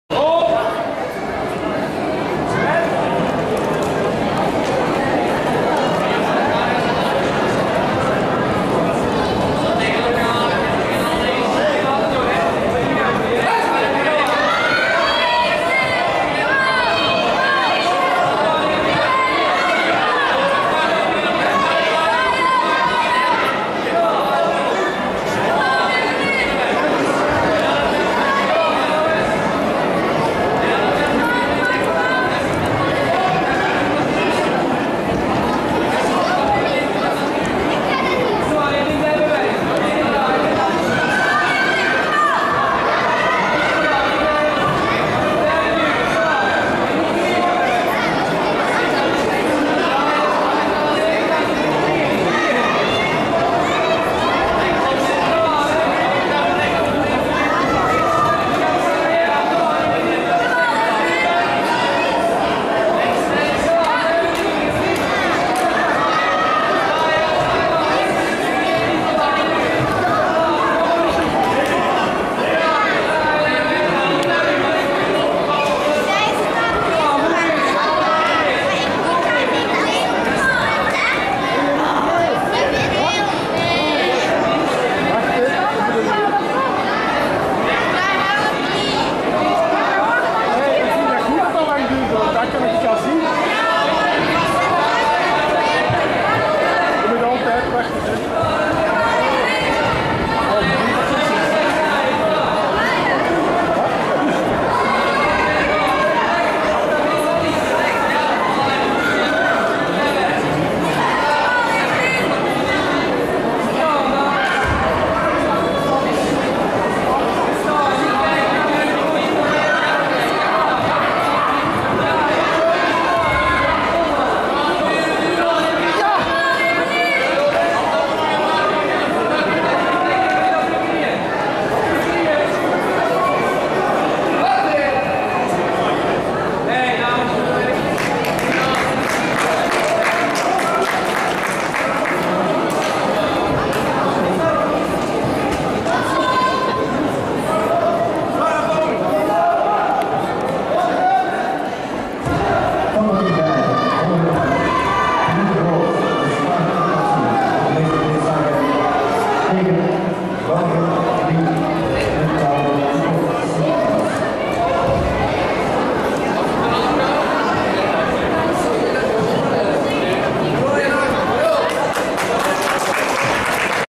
OOVK 2014 ??? - Evelyn
Open Oost-Vlaams 09/02/2014 Sporthal De Meerminnen Beveren
Competitievorm groundfight